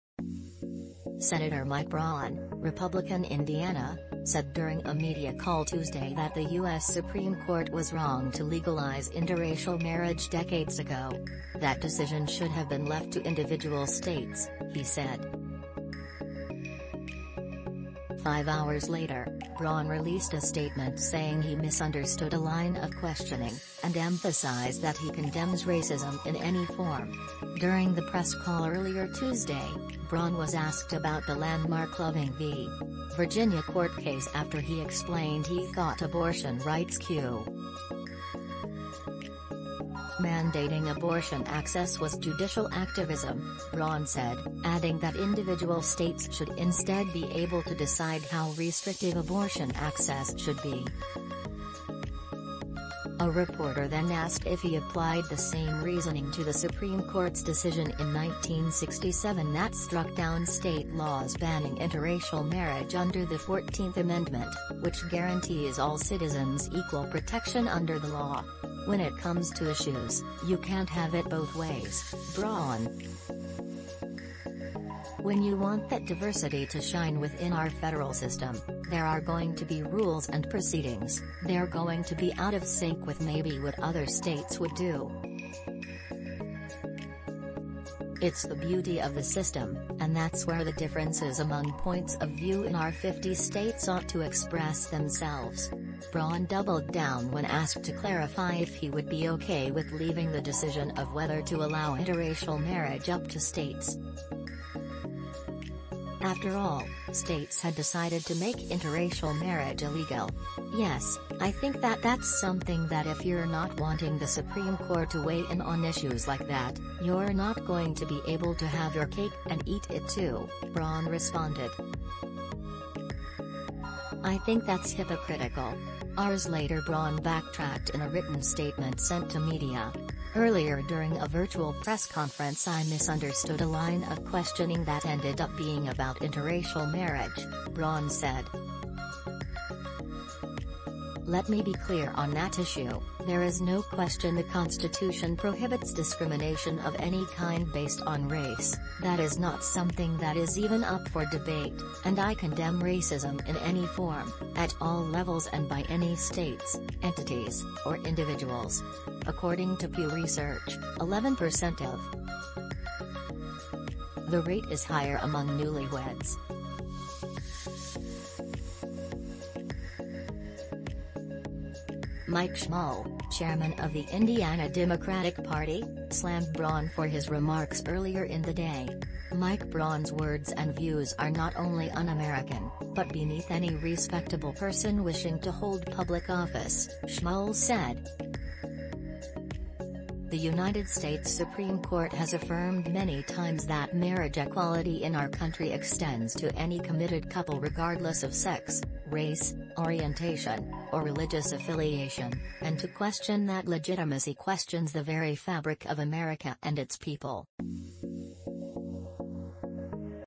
Indiana Sen Braun walks back comments that interracial marriage ruling
Sen. Mike Braun, R-Ind., said during a media call Tuesday that the U. S. Supreme Court was wrong to legalize interracial marriage decades ago. That decision should have been left to individual states, he said. Five hours later, Braun released a statement saying he misunderstood "a line of questioning," and emphasized that he condemns racism "in any form."During the press call earlier Tuesday, Braun was asked about the landmark Loving v. Virginia court case after he explained he thought abortion rights questions should have been left up to the states back when Roe v. Wade was decided in 1973.  Mandating abortion access was judicial activism, Braun said, adding that individual states should instead be able to decide how restrictive abortion access should be. A reporter then asked if he applied the same reasoning to the Supreme Court's decision in 1967 that struck down state laws banning interracial marriage under the 14th amendment, which guarantees all citizens equal protection under the law."When it comes to issues, you can't have it both ways," Braun said. "When you want that diversity to shine within our federal system, there are going to be rules and proceedings, they're going to be out of sync with maybe what other states would do. It's the beauty of the system, and that's where the differences among points of view in our 50 states ought to express themselves."Braun doubled down when asked to clarify if he would be OK with leaving the decision of whether to allow interracial marriage up to states. After all, states had decided to make interracial marriage illegal."Yes, I think that that's something that if you're not wanting the Supreme Court to weigh in on issues like that, you're not going to be able to have your cake and eat it too," Braun responded. "I think that's hypocritical."Hours later Braun backtracked in a written statement sent to media.“Earlier during a virtual press conference I misunderstood a line of questioning that ended up being about interracial marriage," Braun said. "Let me be clear on that issue — there is no question the Constitution prohibits discrimination of any kind based on race, that is not something that is even up for debate, and I condemn racism in any form, at all levels and by any states, entities, or individuals.”According to Pew Research, 11% of all married adults in the U. S. had a spouse who was a different race or ethnicity from them in 2019. The rate is higher among newlyweds. Mike Schmuhl, chairman of the Indiana Democratic Party, slammed Braun for his remarks earlier in the day."Mike Braun’s words and views are not only un-American, but beneath any respectable person wishing to hold public office," Schmuhl said. "The United States Supreme Court has affirmed many times that marriage equality in our country extends to any committed couple regardless of sex, race, orientation, or religious affiliation, and to question that legitimacy questions the very fabric of America and its people.